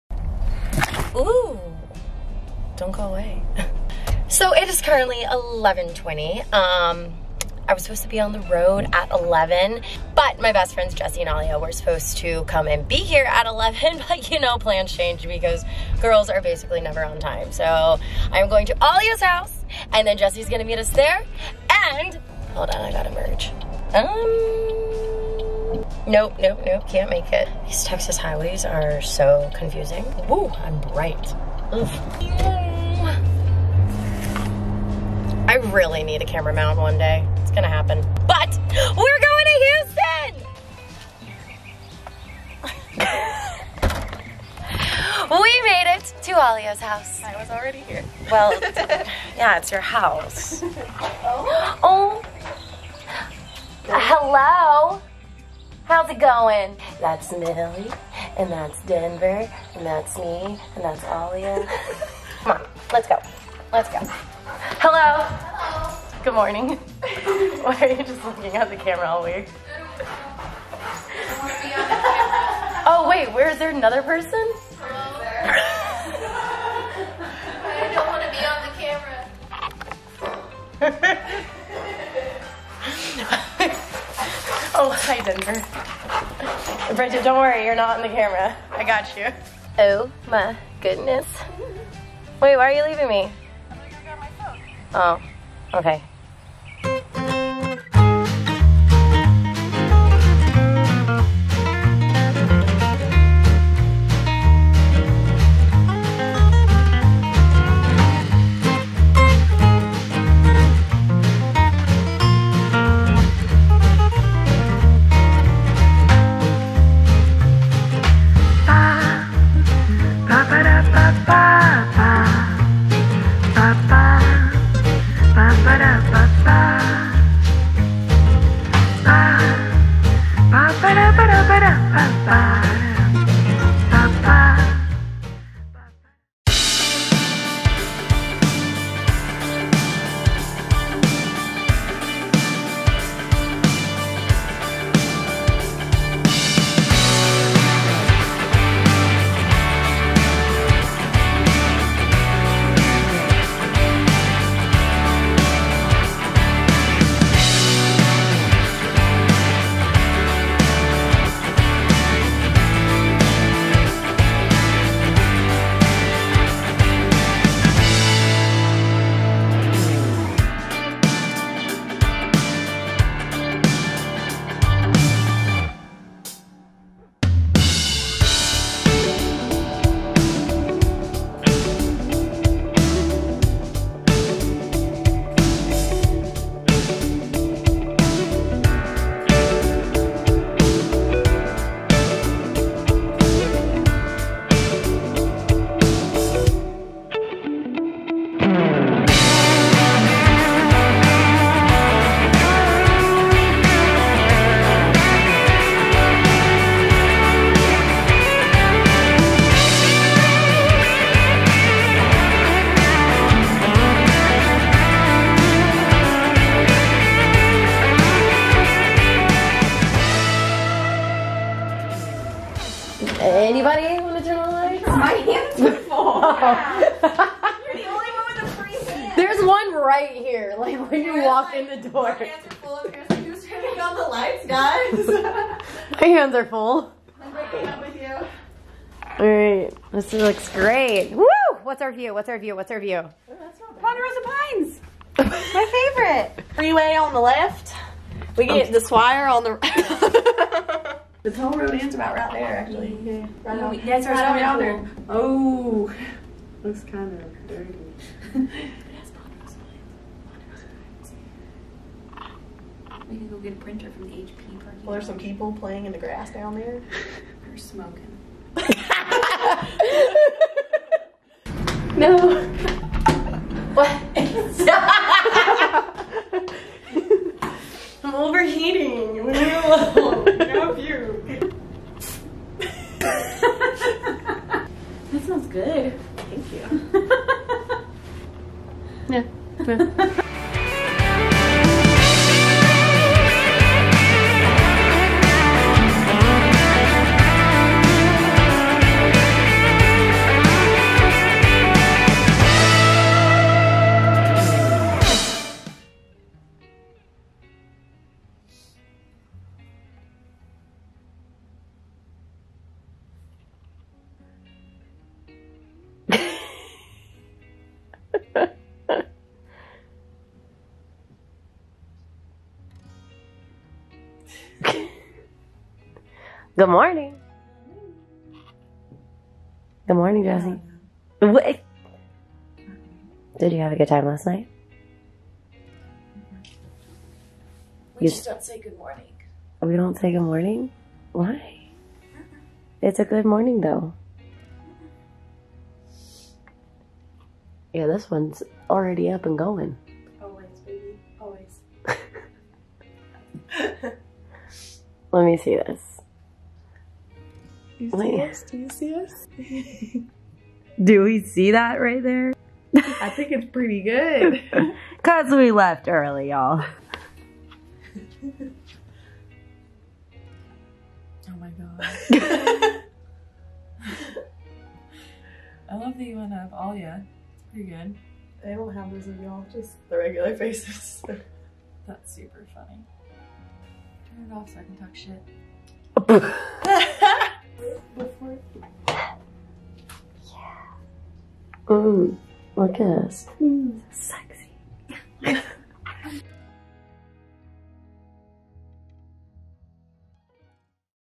Dancing in Houston!!
My besties in TEXAS! Jessie, Alia, and I took a trip to Houston to help shoot a video and learn some moves with @countrydancex

We had an absolute blast even though it rained. Can't wait to share more with y'all!